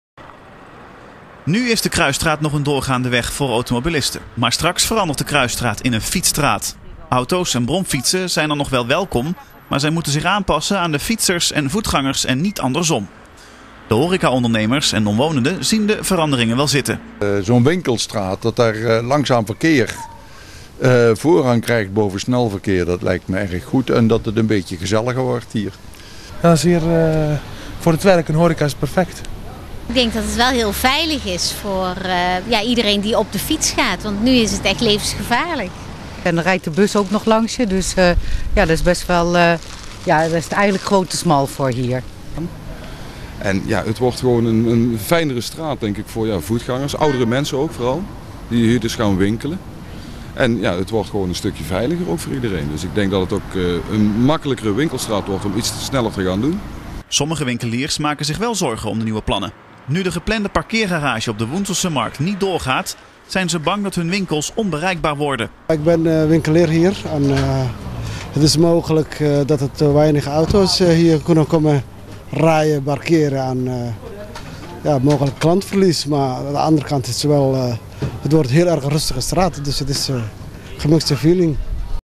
Wat vindt Eindhoven van de Kruisstraat? - 16-06-2010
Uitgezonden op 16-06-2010

Wat vindt Eindhoven van de plannen om van de Kruisstraat een fietsstraat te maken?